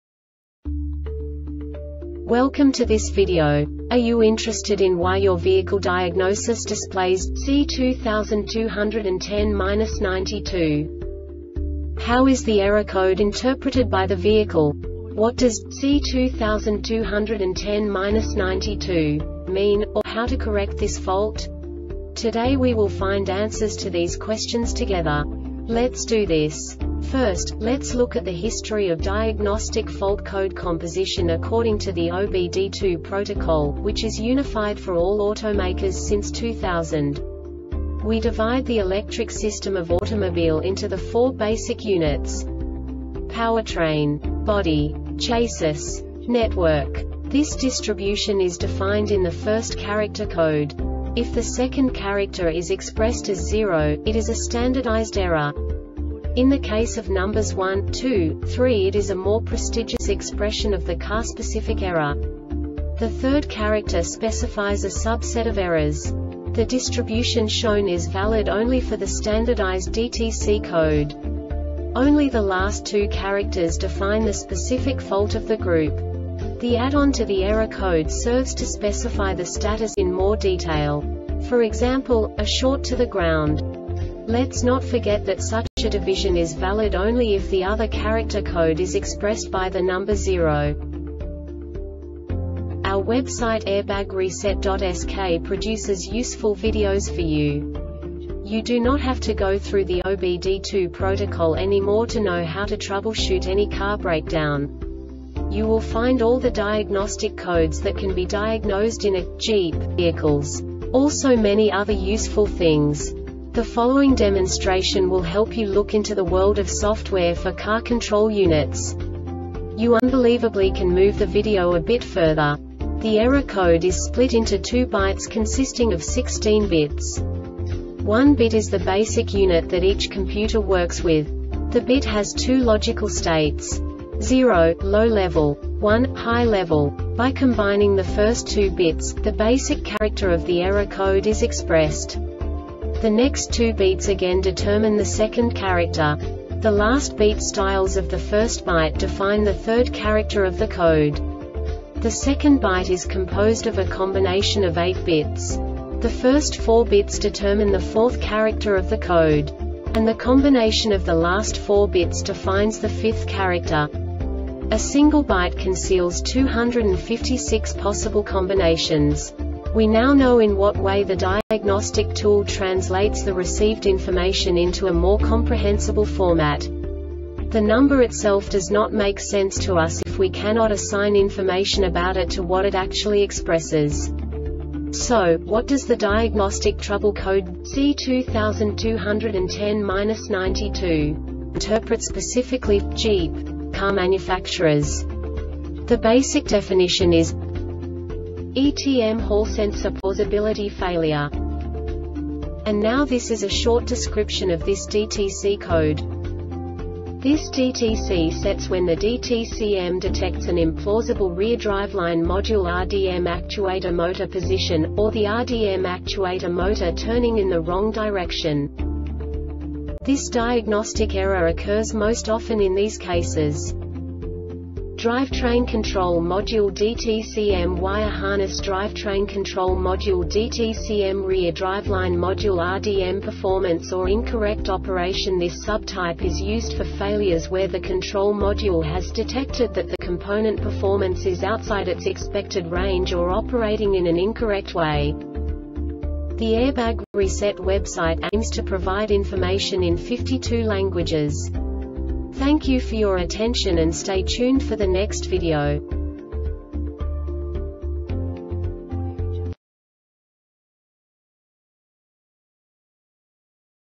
DTC Jeep C2210-92 Short Explanation
Contents:
0:21 Basic DTC analysis according to OBD2 protocol standard.
1:48 Insight into programming
2:58 A diagnostically understandable description of the Diagnostic Trouble Code
3:05 Basic error definition

Make:
Jeep
Code:
C2210-92
Definition:
ETM Hallsensor Plausibility Failure
Description:
This DTC sets when the DTCM detects an implausible Rear Driveline Module (RDM) Actuator Motor position, or the RDM Actuator Motor turning in the wrong direction
Cause:
Drivetrain control module (DTCM) wire harnessDrivetrain control module (DTCM)Rear driveline module (RDM)
Failure Type: